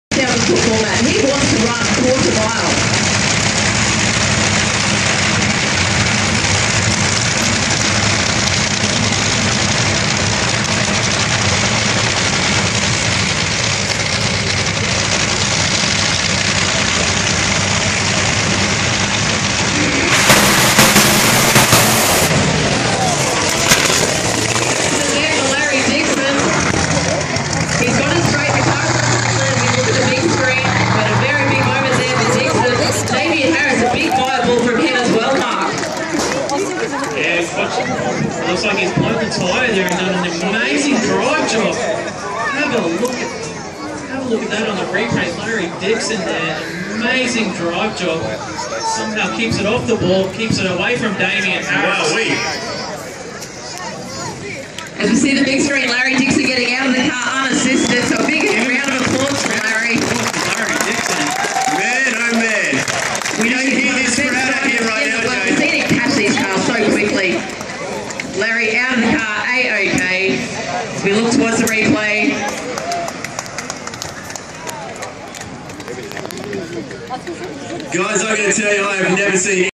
Larry Dixon top fueler 360 willowbank.
How to 360 a top fueler and still run an 8.77.